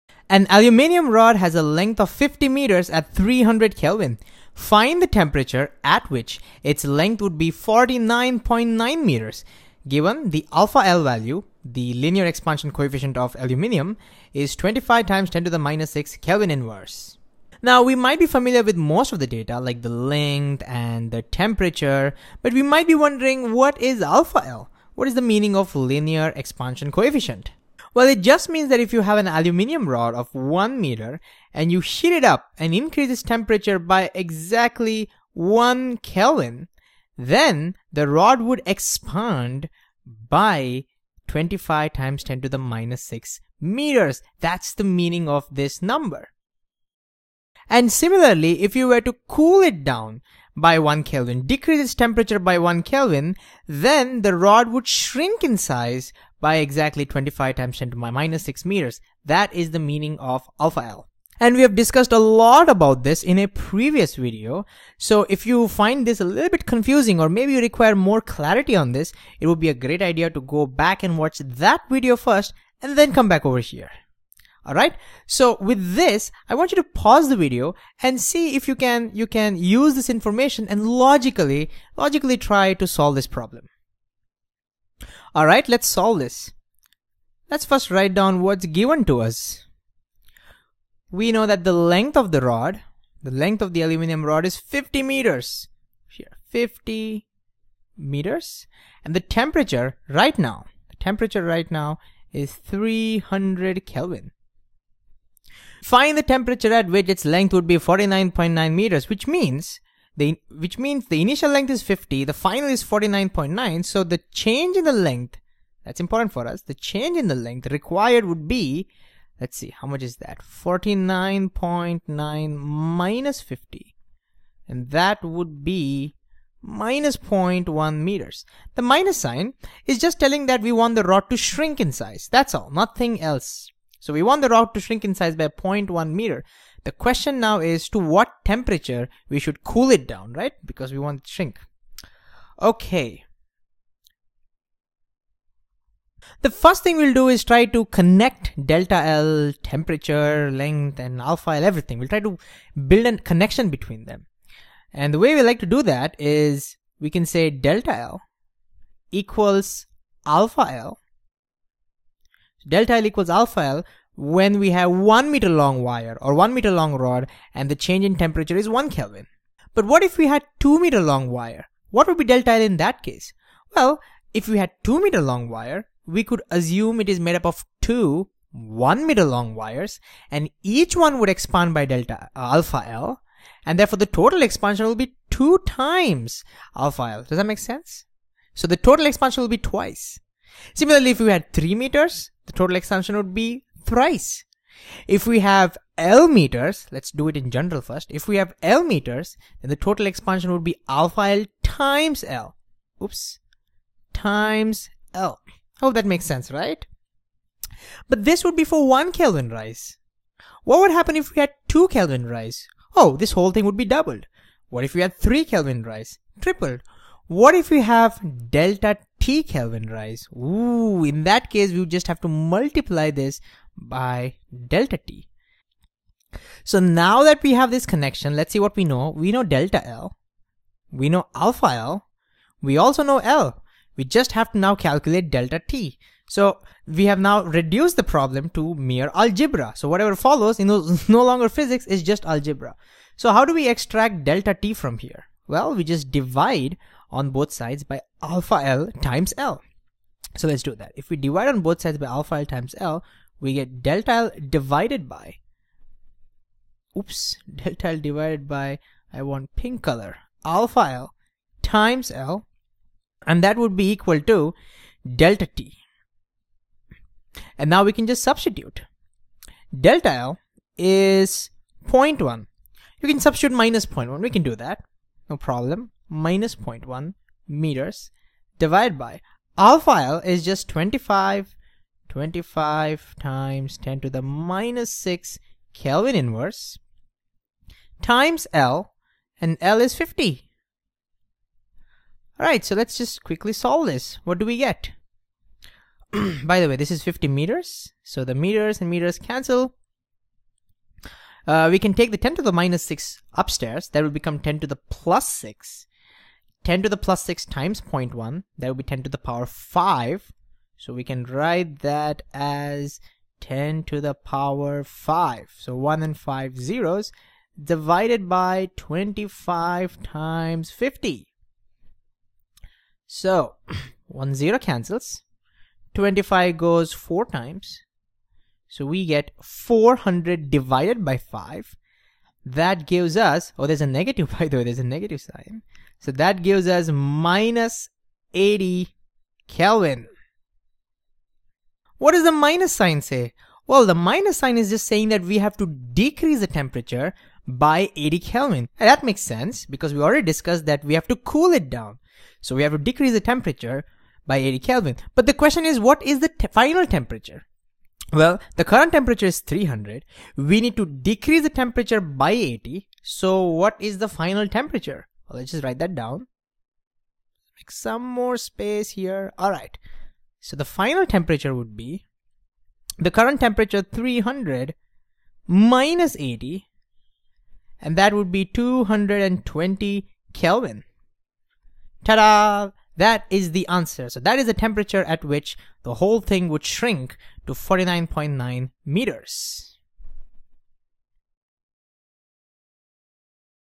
Linear expansion numerical | Class 11 (India) | Physics | Khan Academy
Let's put our linear expansion coefficient to action.
In this video we will explore a numerical on linear expansion and see, step by step, how to tackle such problems.